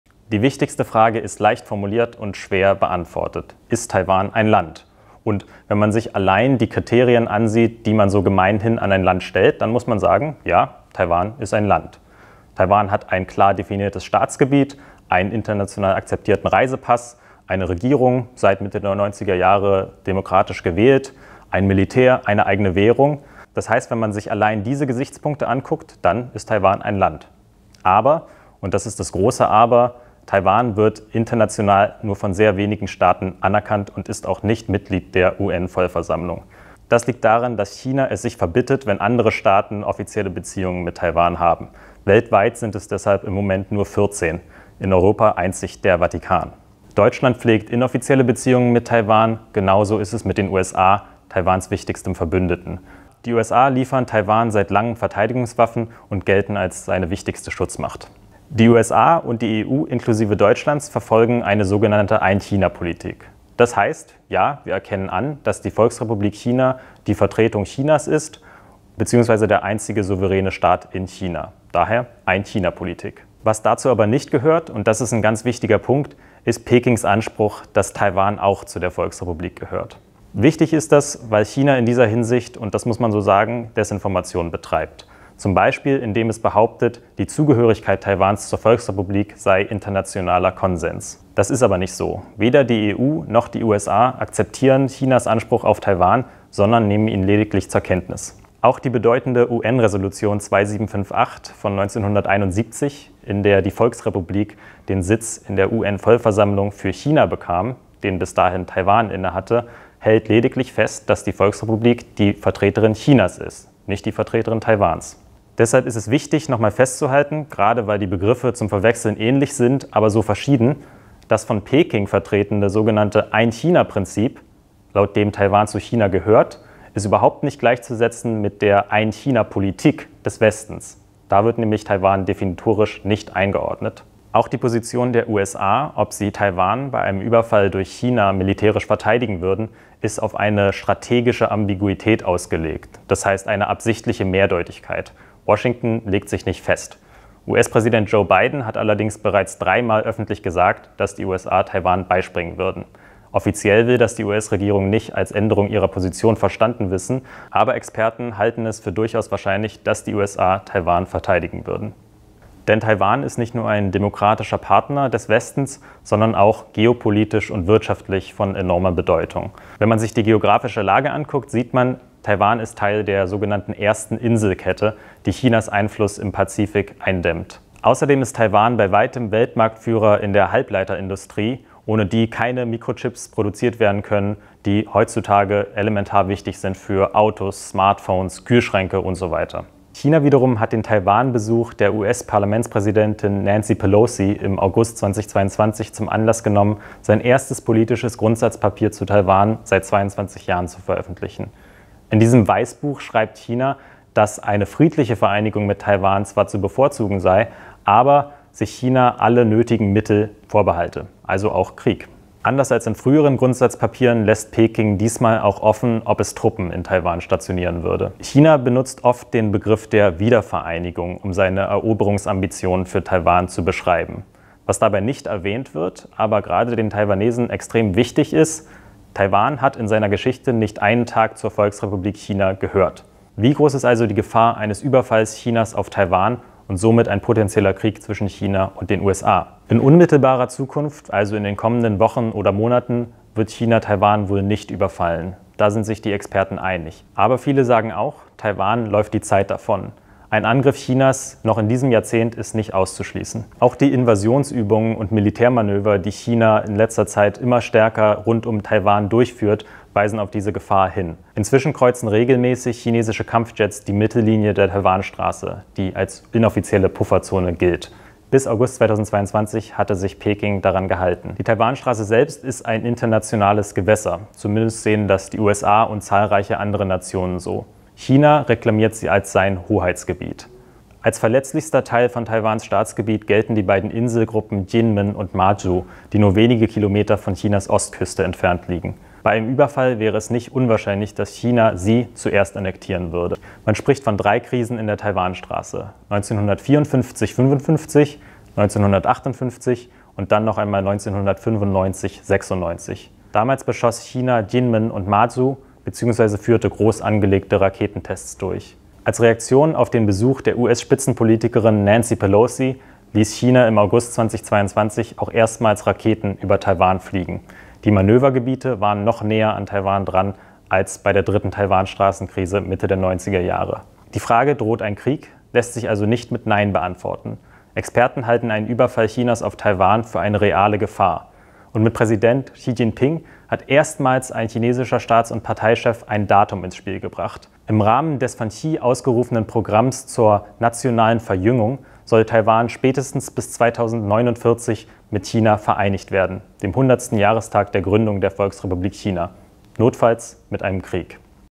China-Taiwan-Konflikt: Droht ein Krieg?
Taiwan – eine blühende Demokratie in Ostasien, knapp 24 Millionen Einwohner und eine Wirtschaftsleistung so hoch wie die der Schweiz.

Der kleine Inselstaat im Pazifik, ungefähr 180 Kilometer von der Ostküste Chinas entfernt, müsste uns in Deutschland vermeintlich gar nicht so sehr interessieren. Aber Taiwan ist auch ein zentraler Schauplatz der Weltpolitik. Manche Experten sagen sogar: der wichtigste Schauplatz der kommenden Jahrzehnte.

Das faktisch unabhängige Taiwan wird immer stärker von seinem Nachbarland China bedroht, das die Inselrepublik als Teil seines Territoriums betrachtet – obwohl Taiwan nie zur Volksrepublik China gehört hat.

Kriegsschiffe, Kampfjets, Raketen und immer militantere Rhetorik aus Peking machen es wichtiger denn je, diesen Konflikt zu begreifen. Denn wenn sich China irgendwann zu einem Überfall auf Taiwan entschiede, was es explizit nicht ausschließt, könnten sich bald die beiden größten Militärmächte der Welt in einem Krieg gegenüberstehen: China und die USA.

Eine kurze Einführung von Tagesspiegel-Redakteur Cornelius Dieckmann.